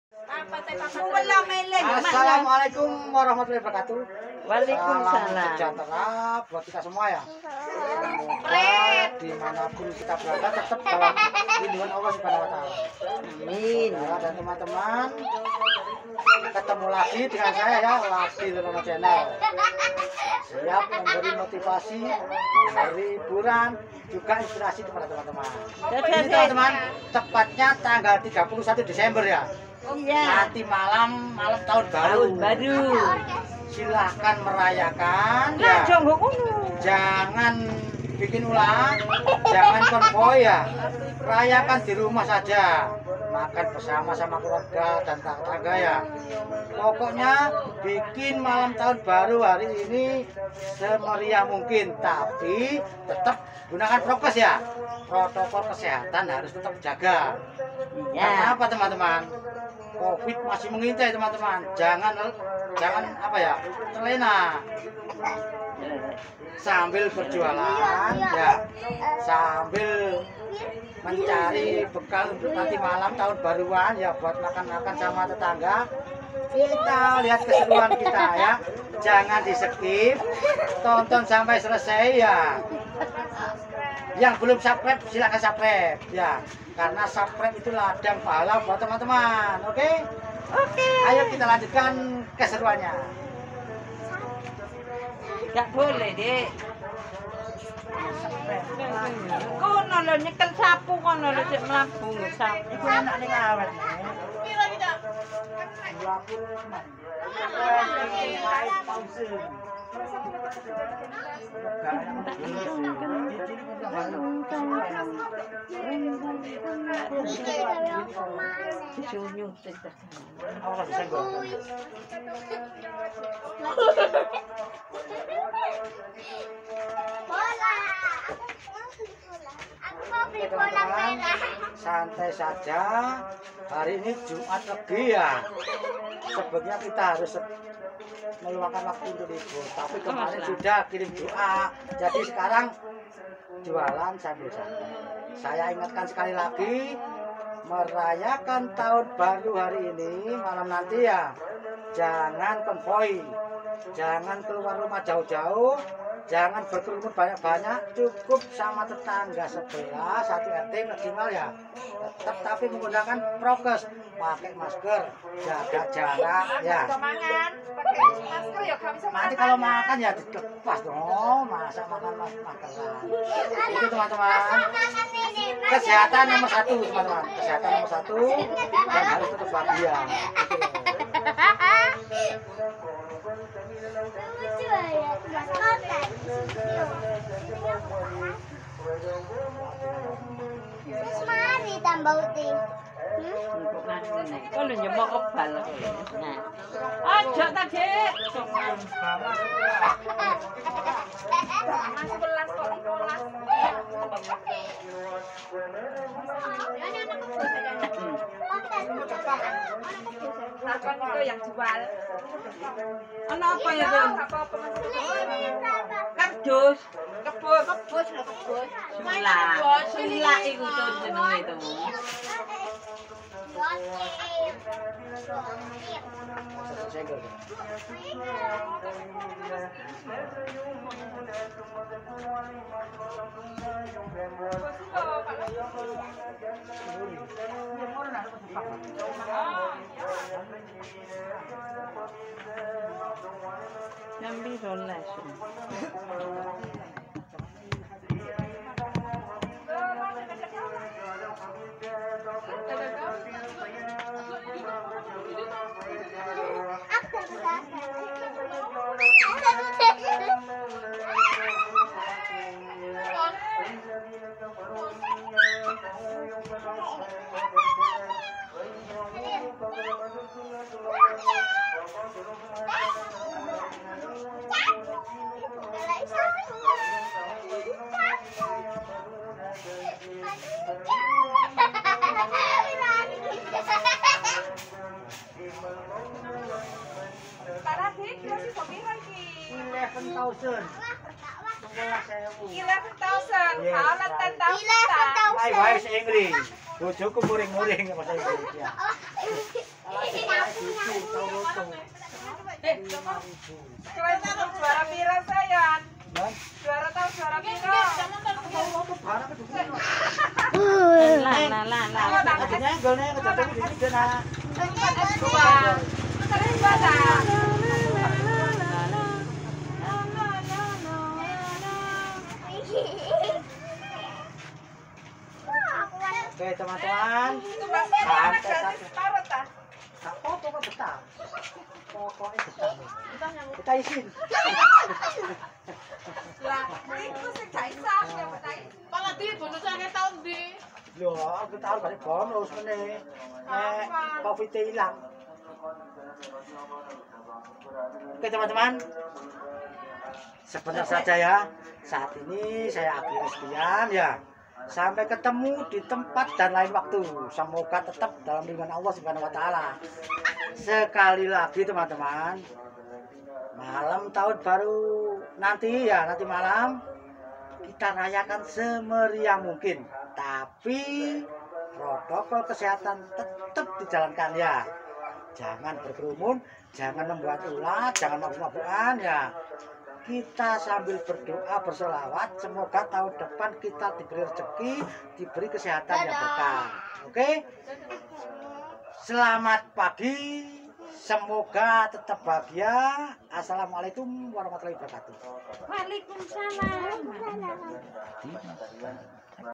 SELAMAT TAHUN BARU 2022 DARI PEDAGANG PERABOT KELILING.
Selamat tahun baru 2022 saya ucapkan kepada saudara dan teman2 dimanapun berada. Silahkan merayakan malam tahun baru 2022, tetapi kesehatan dan keselamatan no 1.Jangan konvoi, jangan mabuk mabukan, jangan melakukan yang menyusahkan dan mengganggu orang lain dan diri sendiri. Selamat tahun baru 2022 semoga kita semua diberikan kesehatan dan rejeki yg berkah.